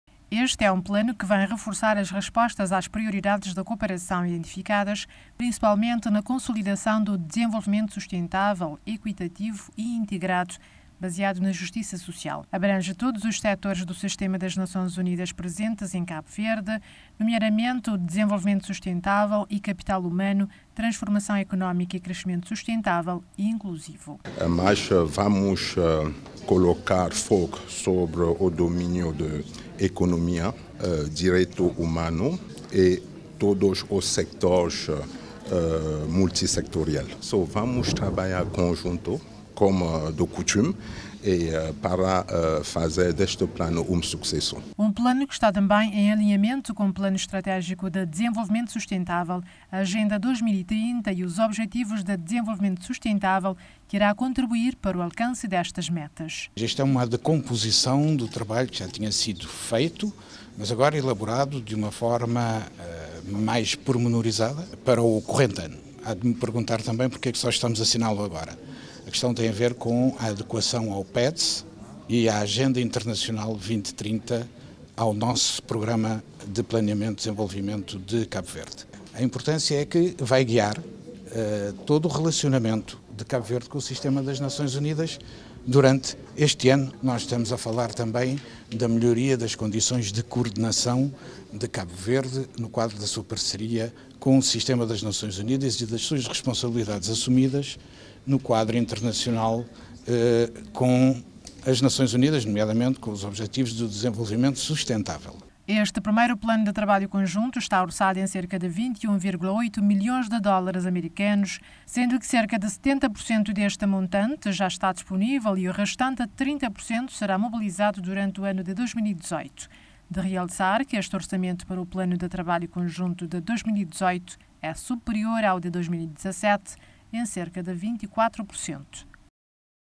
UNDAF 2018 2022
Governo e Sistema das Nações Unidas em Cabo Verde assinam primeiro Plano de Trabalho Conjunto do Quadro de Cooperação da ONU para o Desenvolvimento.